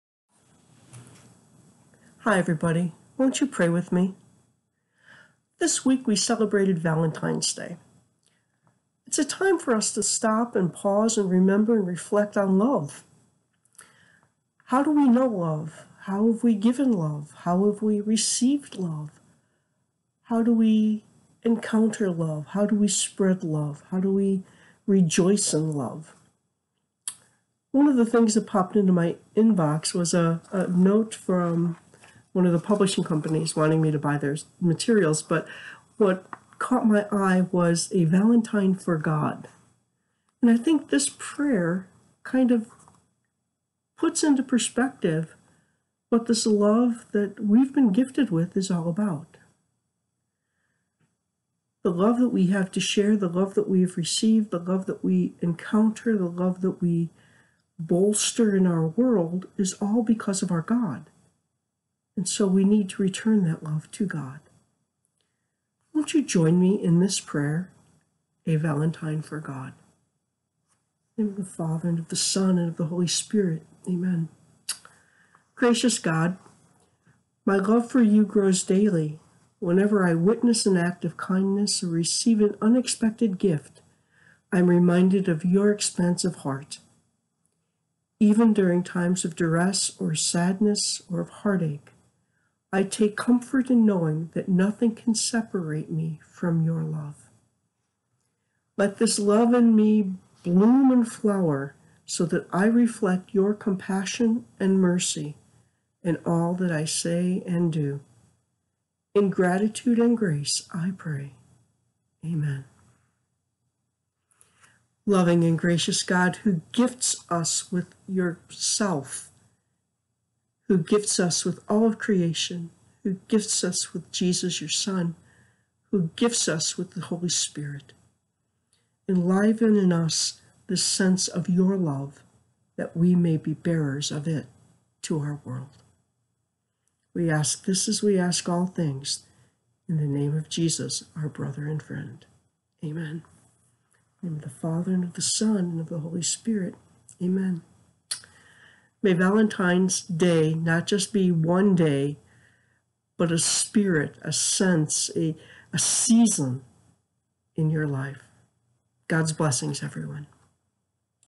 Won't You Pray With Me? 2-16-22
A moment of prayer and reflection with Dr. Cheryl Fournier, Pastoral Associate for Life-Long Faith Formation, Sr. Pius X Catholic Church, Urbandale, IA.